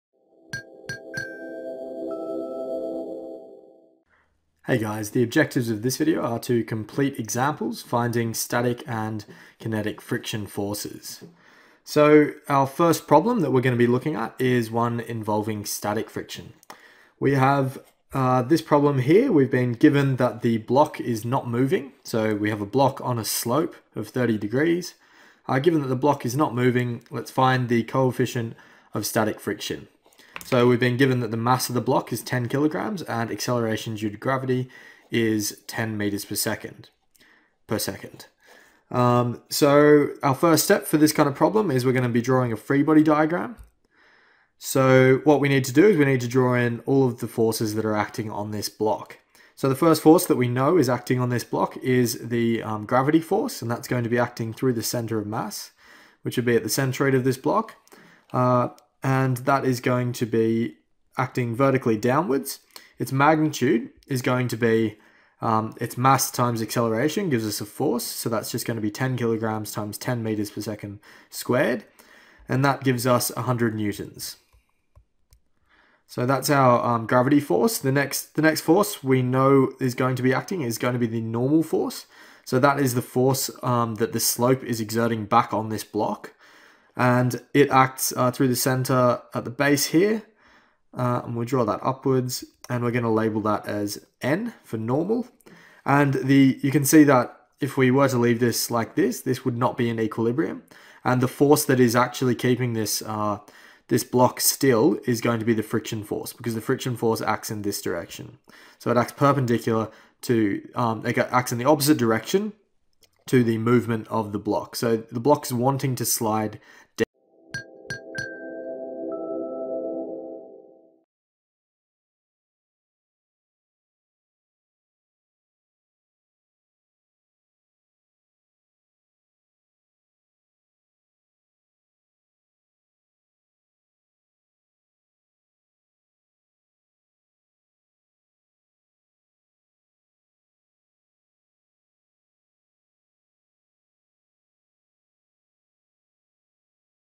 Static Friction Example | Statics
for more FREE video tutorials covering Engineering Mechanics (Statics & Dynamics)

In this tutorial, the video presents a comprehensive example of static friction where it asks to find the coefficient of static friction based on the data given. First part of the video vividly illustrates the given problem where it has shown that a block of 10 Kg mass is rest upon on an inclined surface & the slope of the surface is given as 30 degree. The gravitational acceleration is also given as 10m/s2. Moving on, the video draws the FBD showing the actions of gravity force, normal force and friction force. In addition, the required angles of these forces are also shown over the FBD.

Next, the video applies the equilibrium principle and resolves force components into x & y direction and successively computes out the unknown values from the established equations. Later, the video introduces with the equation of static friction overviewed in the previous video & finally determine the coefficient of static friction putting the required values into the equation. Do on hand practices with the tutorial to have better understanding on the workout completed in this lesson.